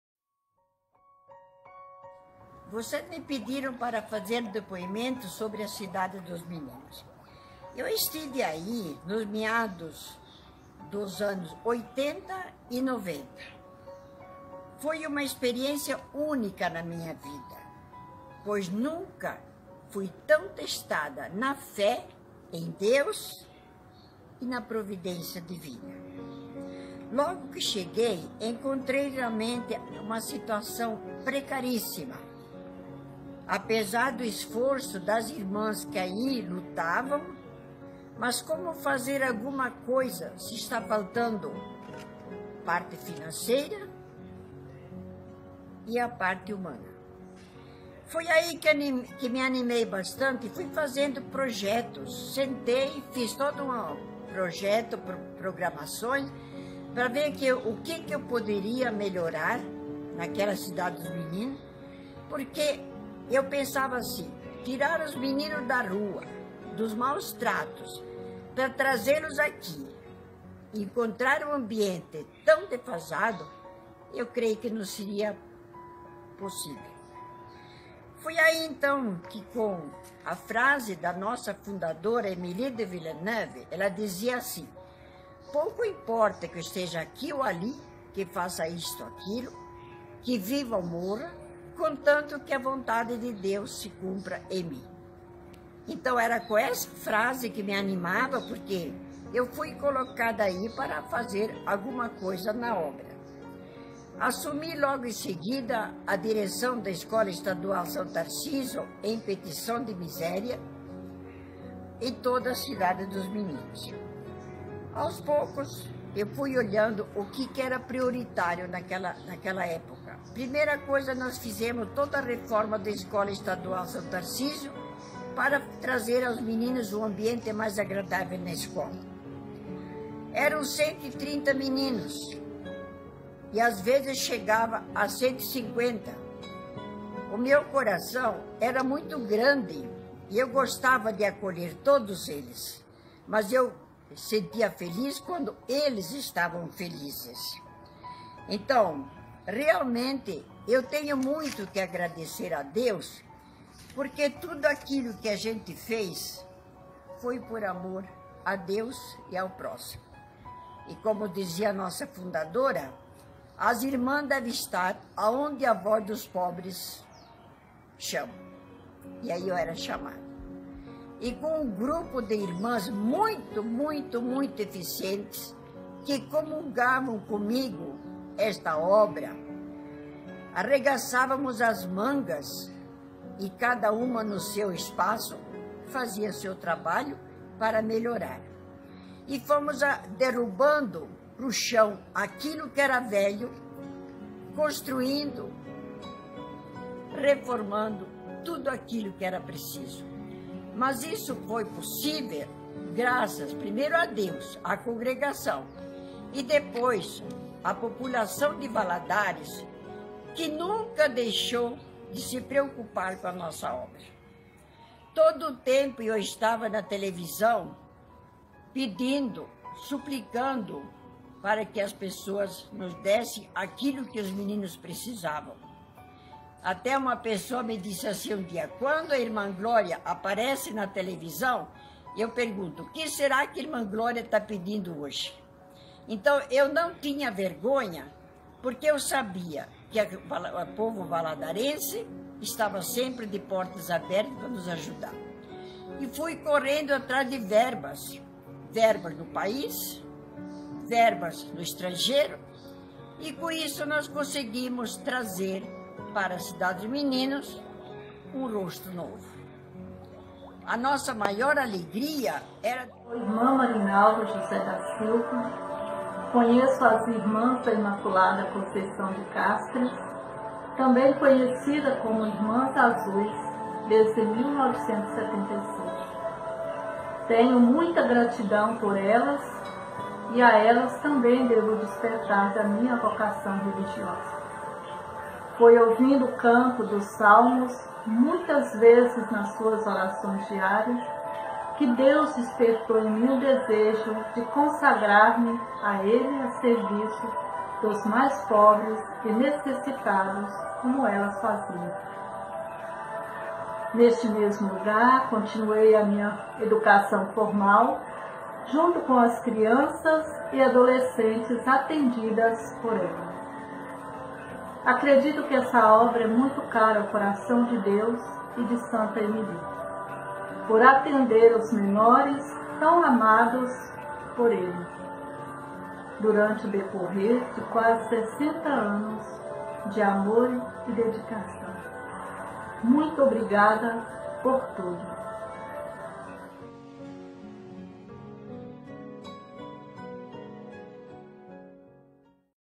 Testemnho de nossas Irmãs Glória Voltoline e Marinalva Silva
Fazendo a Memória da CIDADE dos MENINOS , começamos o dia com nossas Irmãs Glória Voltoline e Marinalva Silva, uma das nossas vocacionadas desta obra!

Geraldo Magalhães
Irmã Glória voltolini, uma super mãe para mim, a conheci na Cidade dos Meninos na década de 90, e para mim como ex interno da Cidade dos Meninos, Irmã Glória foi uma das melhores administradoras daquele lugar. Me lembro que no ano de 1995 quando houve a inauguração do prédio, da capela, dormitório e lavanderia, eu filmei e fotografei, sou testemunha viva de que a Irmã Glória realmente fez o seu melhor em prol daquele lugar. Parabéns Irmã Glória, o meu amor por ti é o mesmo e aumenta a cada dia mais, te amo minha querida, em breve quero revelar e dá-lhe um abraço de filho um beijo no seu coração   te amo.